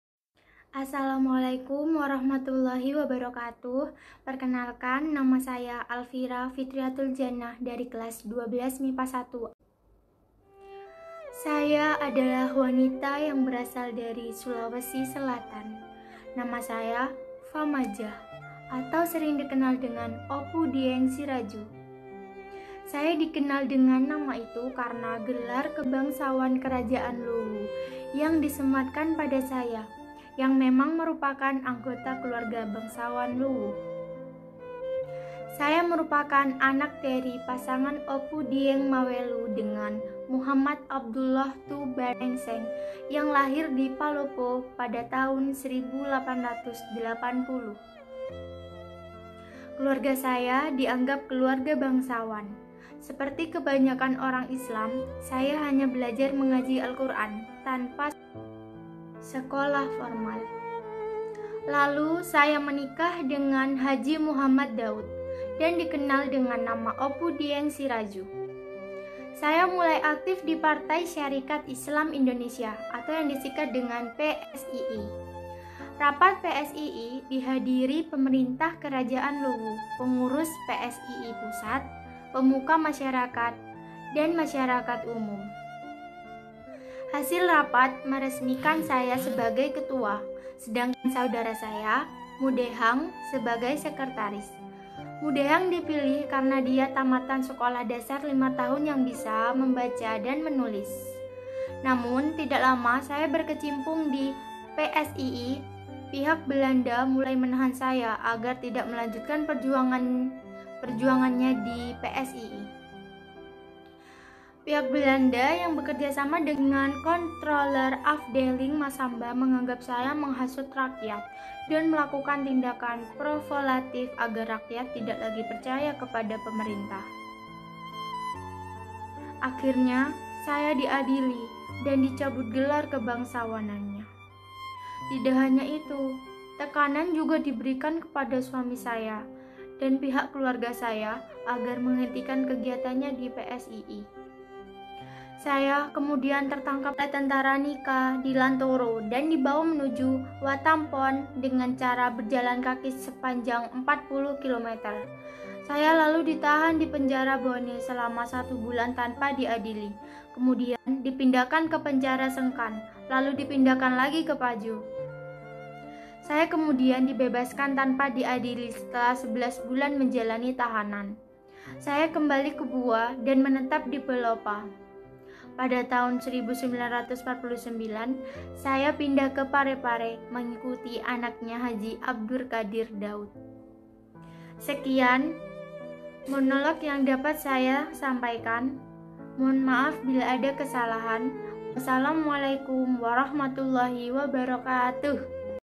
Monolog Opu Dieng Risadju, Alfira Fitriatul Jannah, IPA 1, SMAN 5 Jember
ujian praktik Bahada Indonesia di masa pandemi    Covid -19 Smala Jember